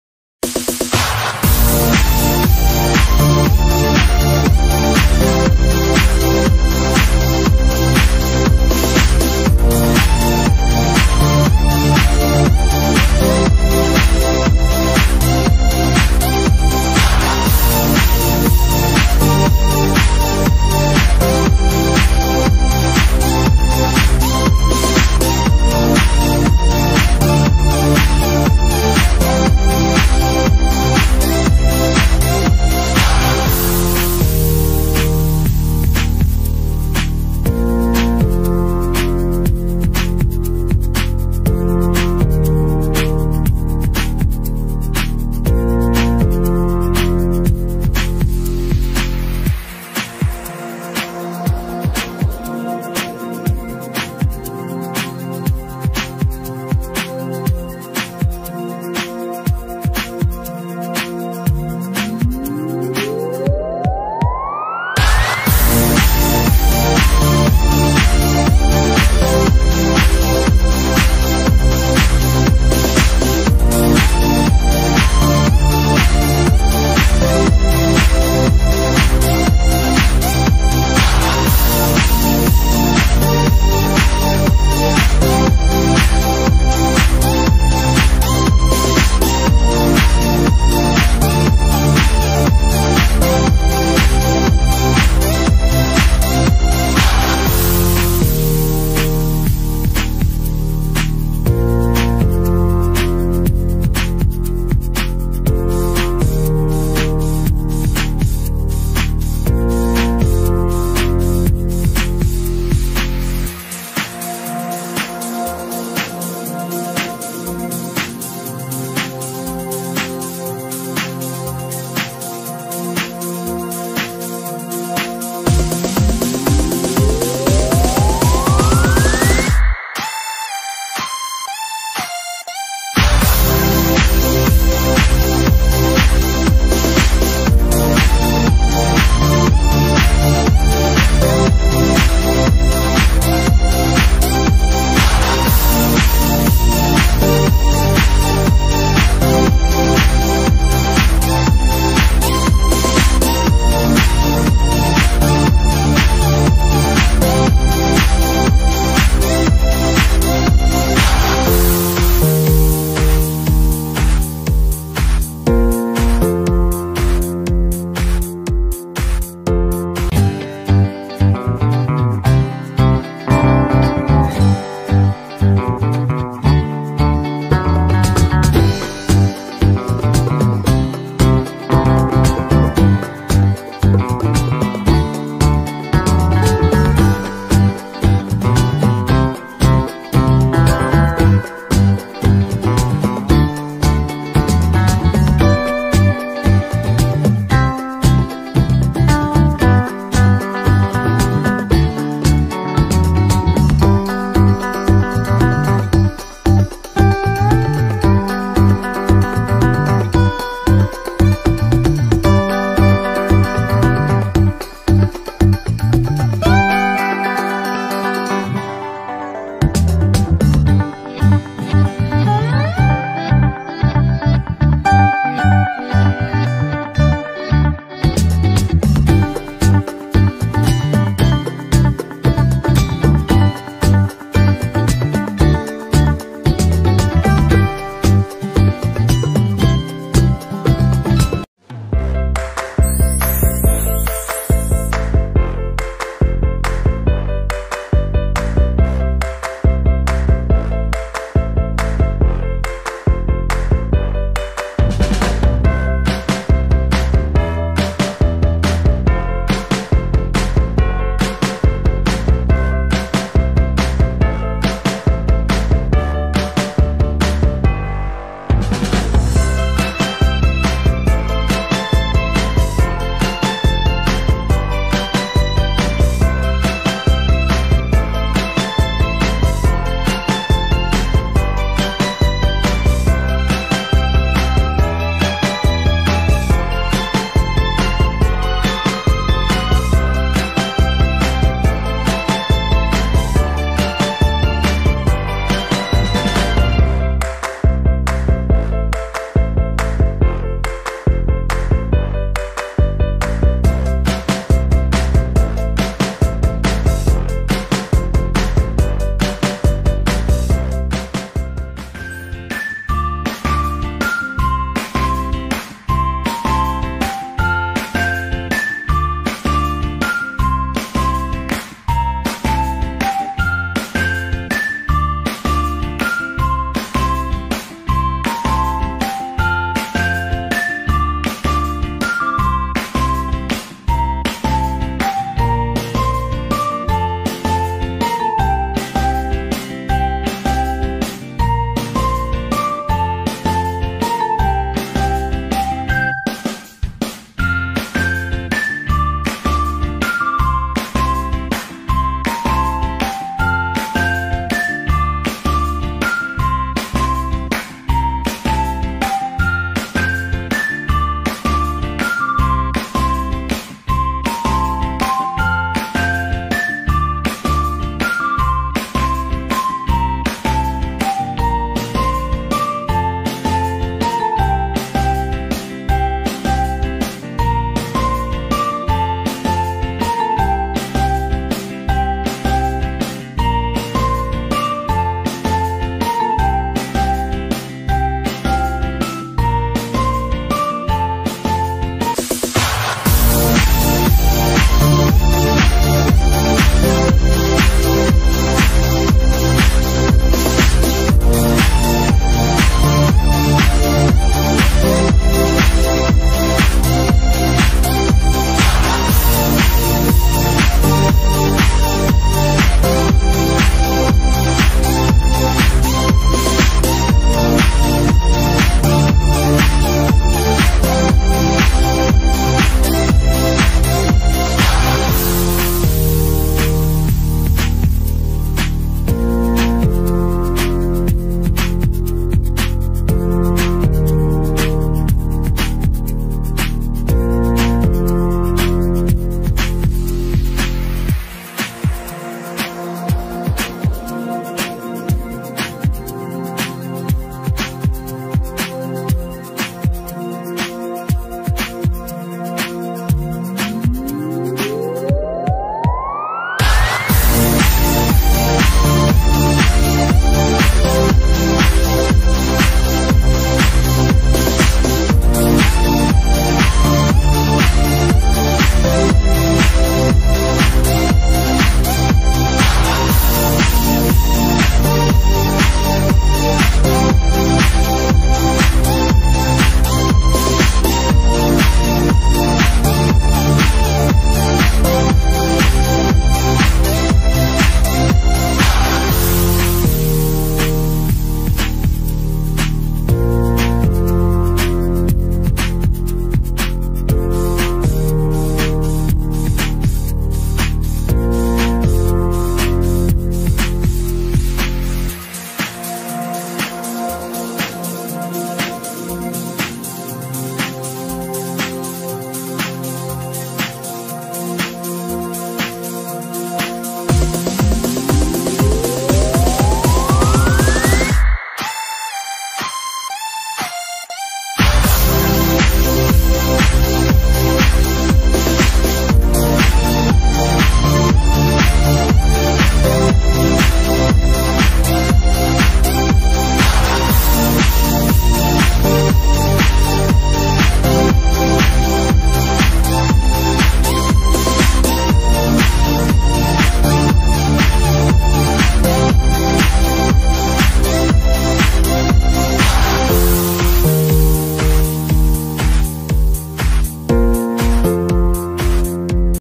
Making Doll Clothes With Balloons #3 3 DIY Dresses For Barbies No Sew No glue [2021]
Satisfying video asmr lollipops candy and chocolate gummy candy Cutting Video Disney